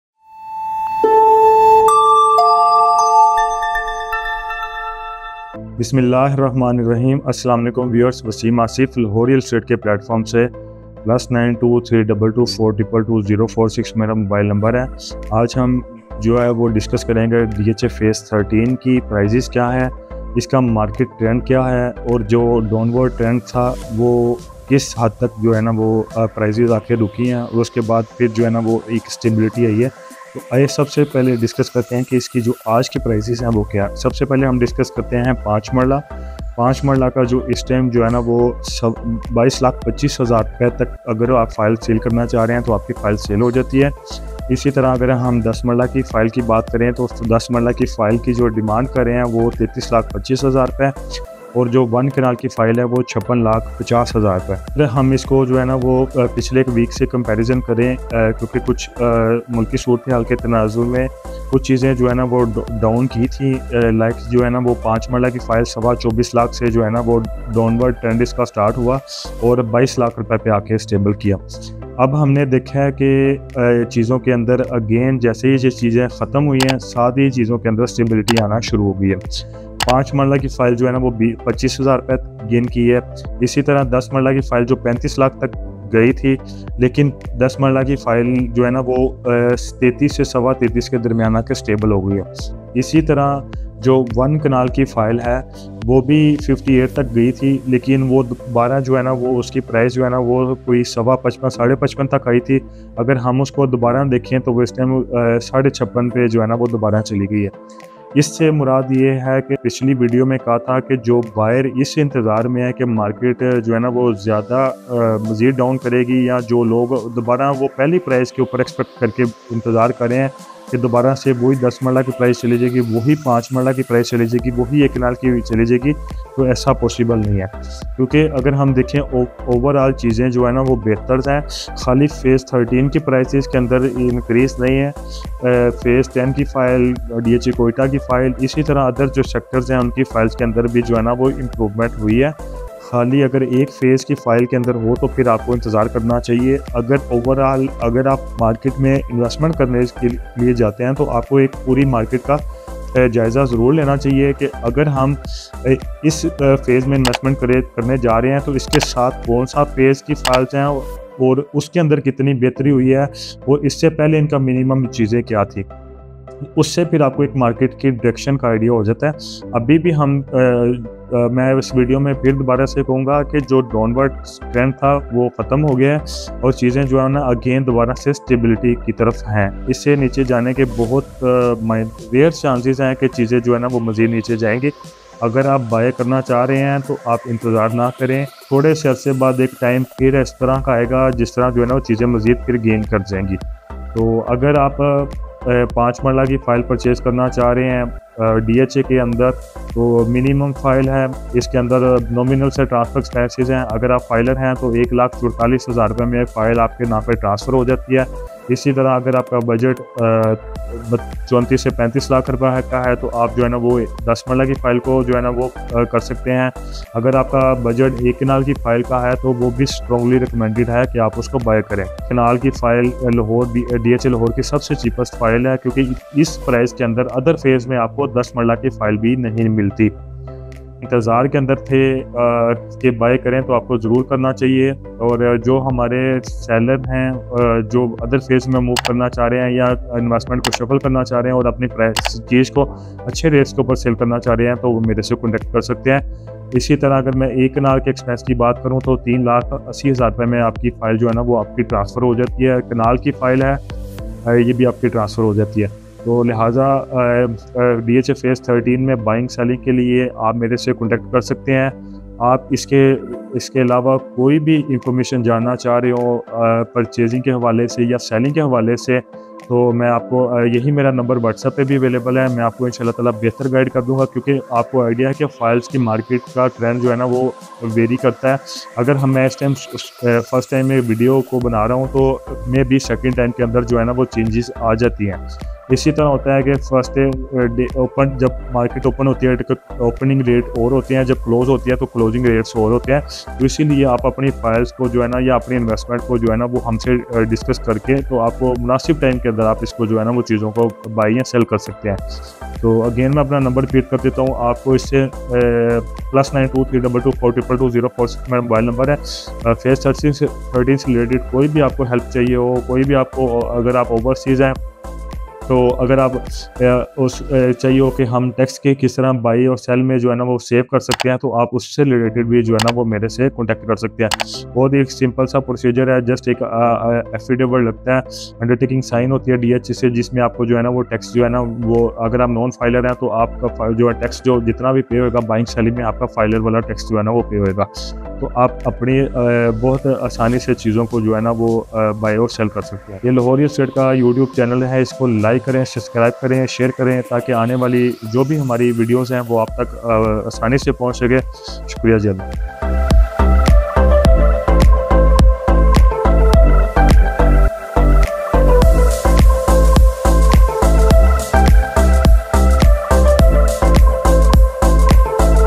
DHA Lahore Phase 13: Market Rates Update & Why It's the Perfect Time to Invest!
Stay updated with the latest market rates of DHA Lahore Phase 13 and discover why now is the perfect time to invest! This video provides an in-depth analysis of current rates, market trends, and the potential for high returns. Don't miss out on this golden investment opportunity!

👉 What You'll Learn:
✔️ DHA Lahore Phase 13 market rates update
✔️ Reasons to invest now
✔️ Insights into future growth potential

LAHORE PAKISTAN